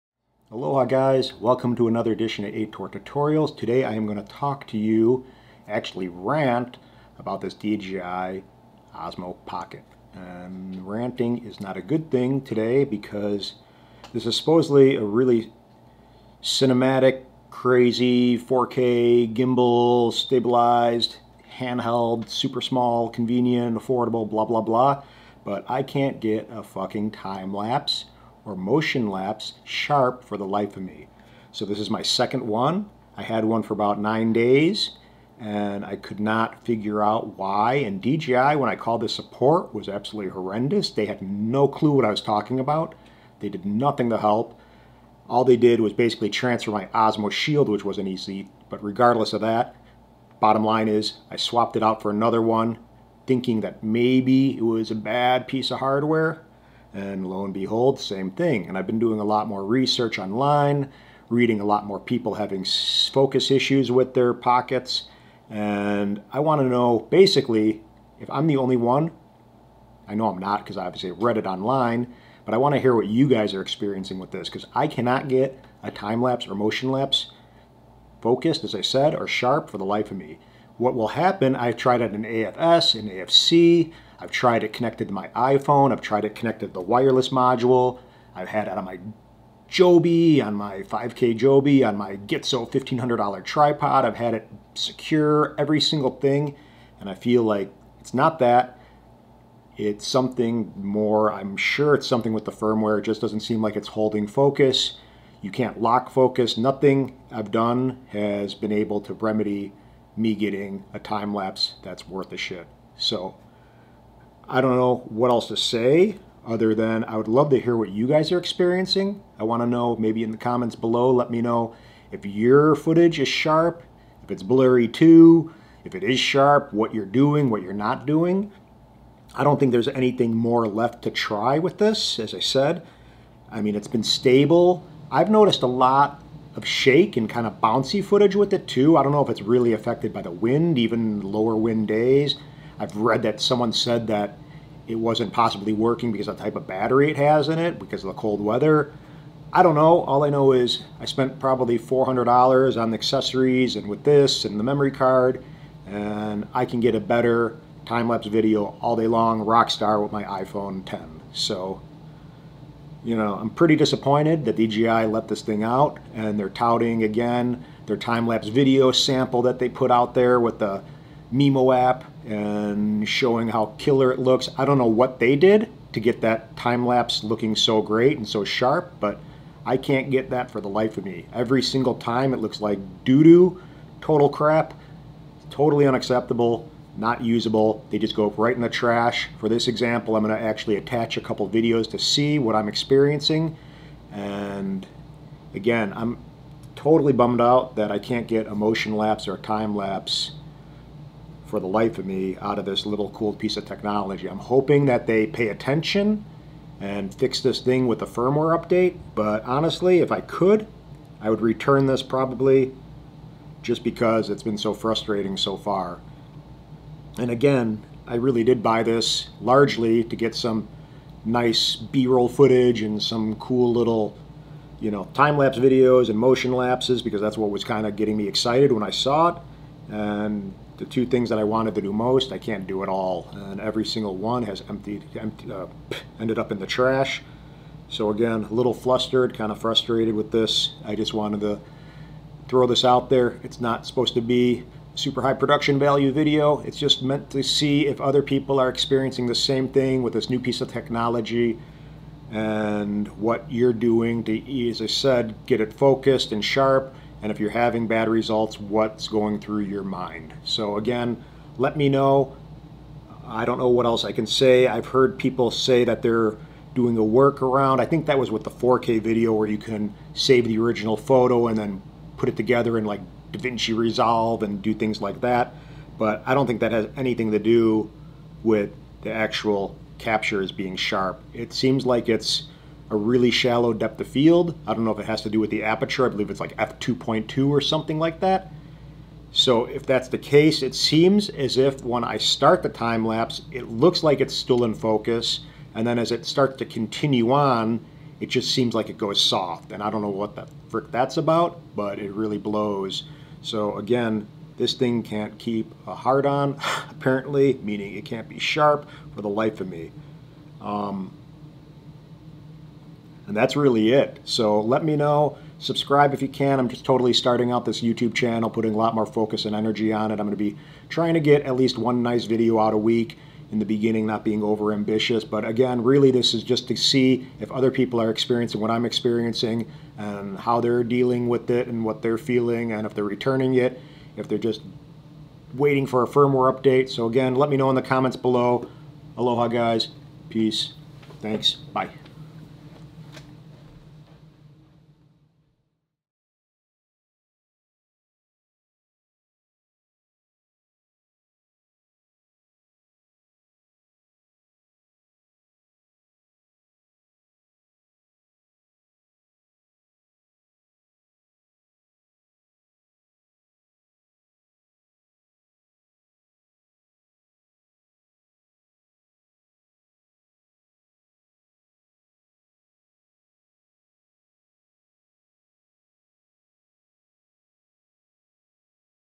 DJI Osmo Pocket Focus Rant!
This is about my experiences with multiple DJI Osmo Pocket's and how I cannot get a focused sharp time lapse or motion lapse for the life of me!  Aloha.